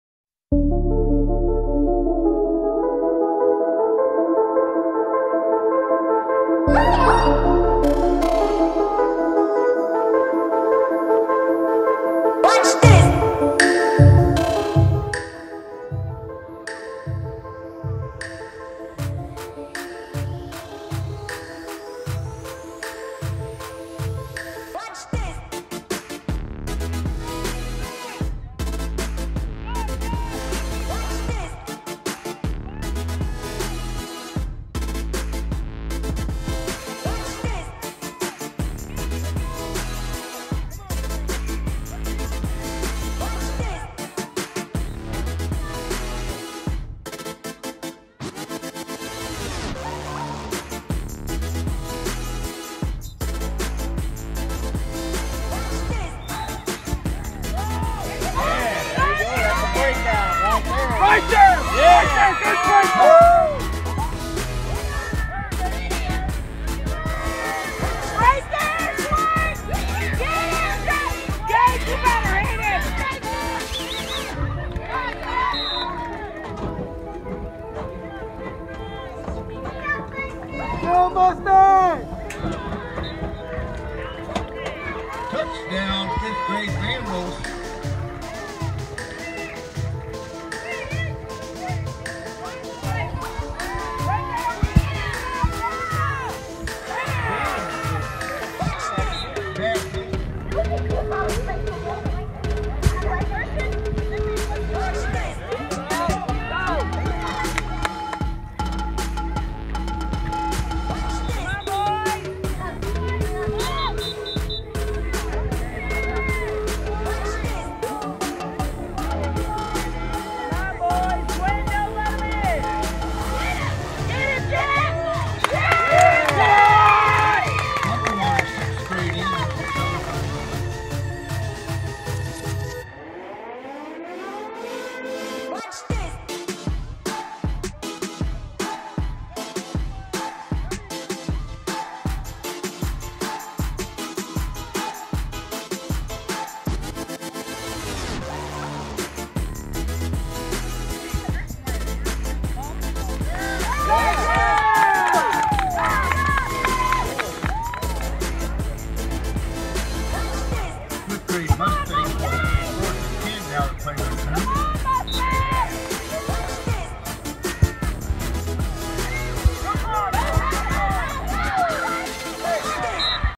Mustangs vs Vandals
In a 20-14 loss to the Vandals the Mustangs proved they could rise to the occasion. The Vandals are big, fast and physical. In turn the Mustangs had play some of their best football. They get a rest and will be back against Roxana

Legally purchased via ITunes